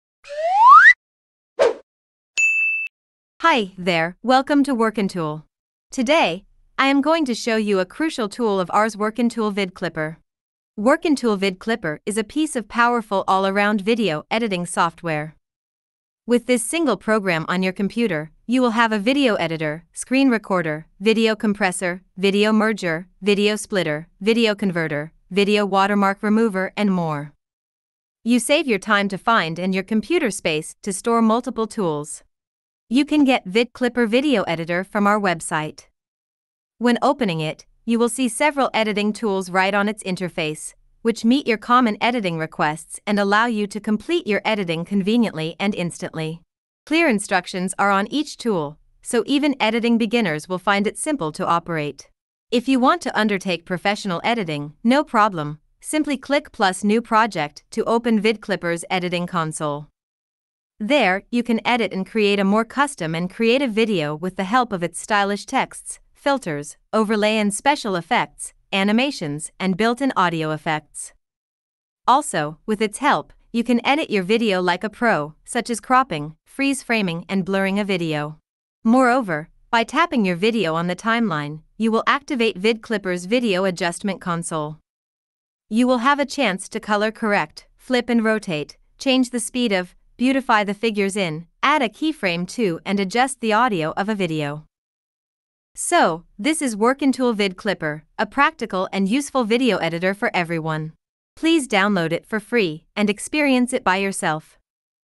WorkinTool VidClipper Makes Your Video Editing Easier and More Interesting
Free, Simple and All-in-One Video Editing Software

VidClipper is a powerful and utterly FREE video editor and screen recorder designed for both beginners and professionals. There are multiple templates for you to make a creative video for your social media, including filters, transitions, overlays, elements and more. Also, it allows you to export a professional video through its post-production functions.